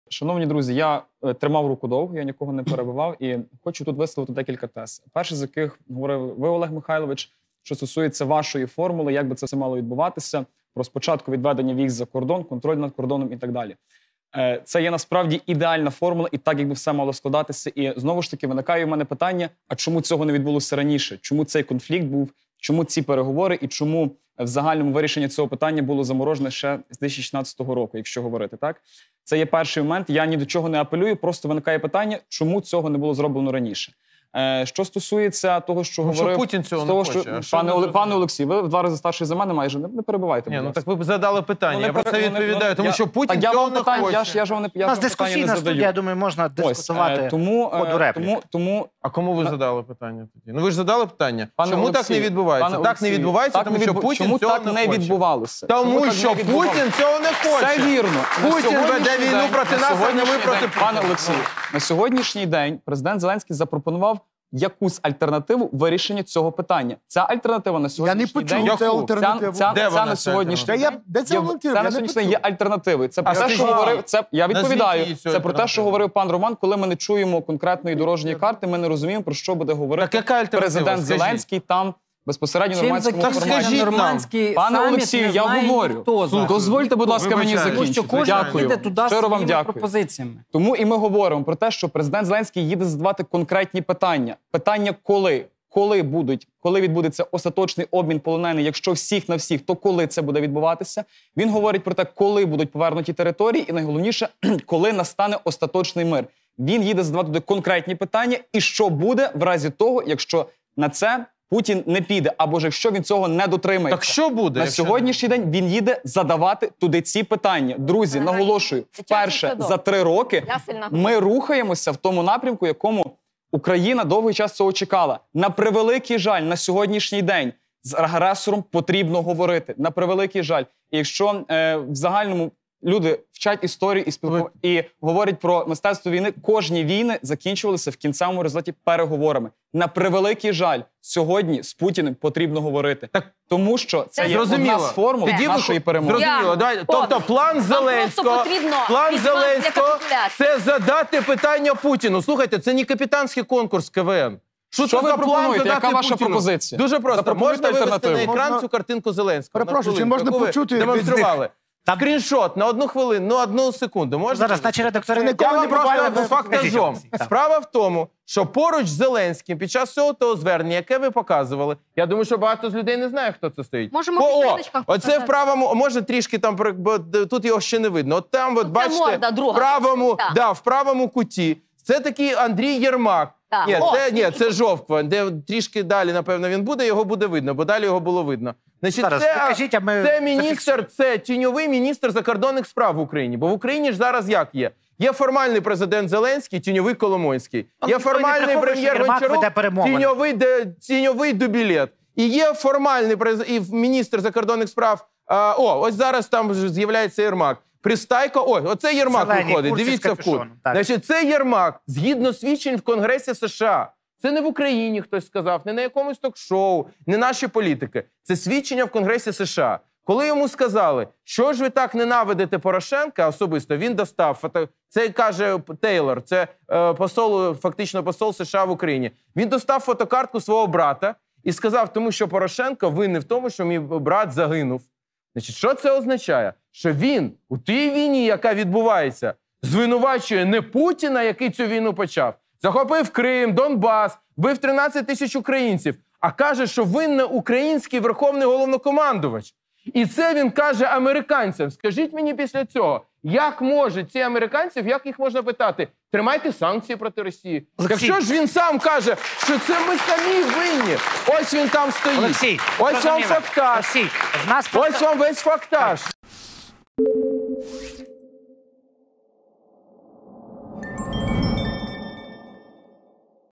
😮Роман Фединяк підтримав зустріч у "нормандському форматі"
😮"З ПУТІНИМ ПОТРІБНО ГОВОРИТИ ❗" - представник ЗЕ-команди у Львові Роман Фединяк підтримав зустріч у "нормандському форматі"